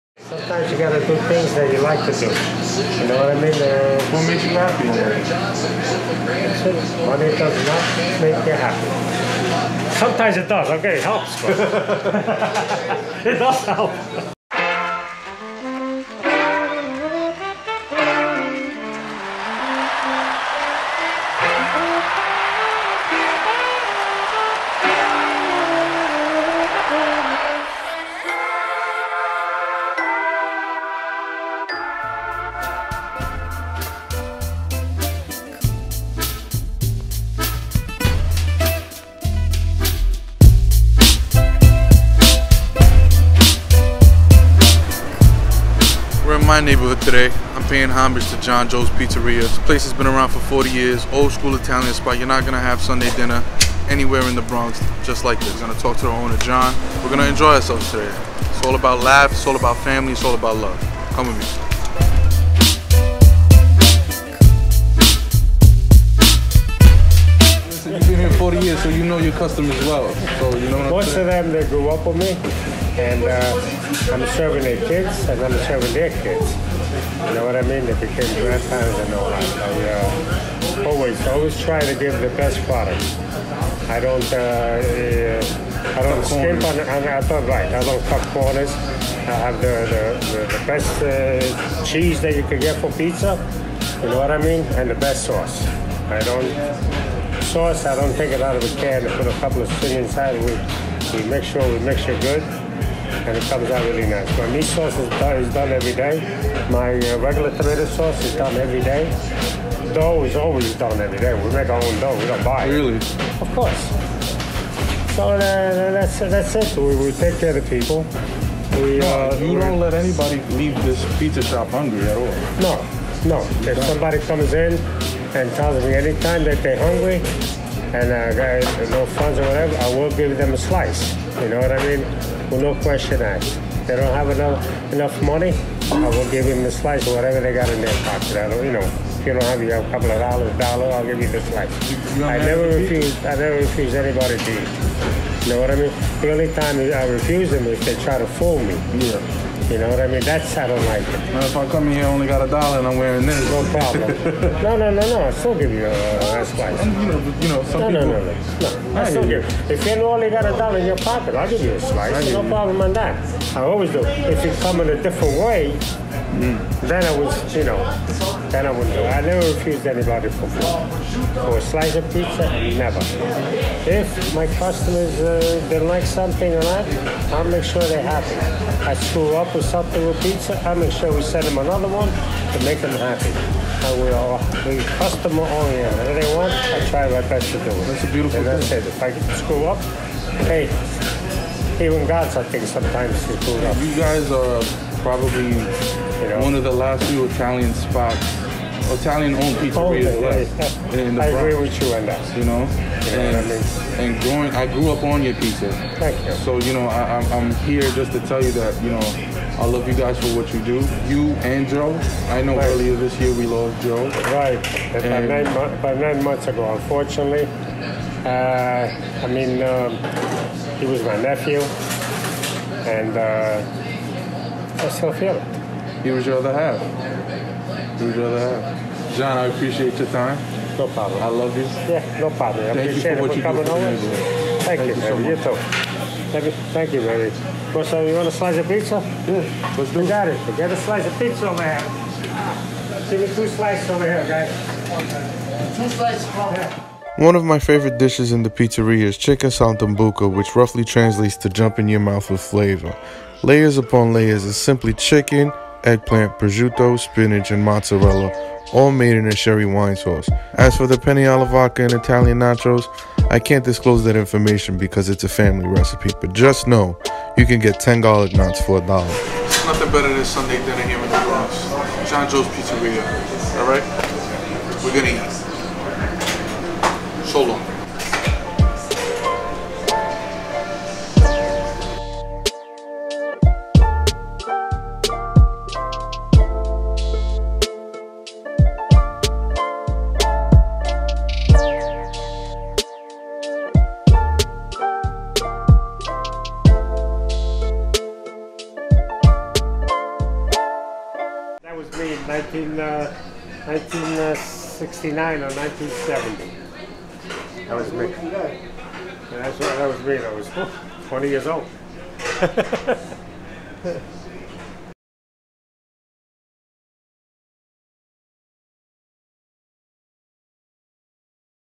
JOHN & JOE'S PIZZERIA
A staple in the community, John & Joe’s pizzeria has been around for nearly 40 years. Many neighborhood restaurants are special in the way they take care of the community and the generations that pass through their doors. John & Joe’s have been taking care of everyone ensuring they do their part by keeping everyone fed. Even during these times, they have managed to stay true to their traditional Italian recipes and continue to serve the community.

IN LOVING MEMORY OF JOE CHIODI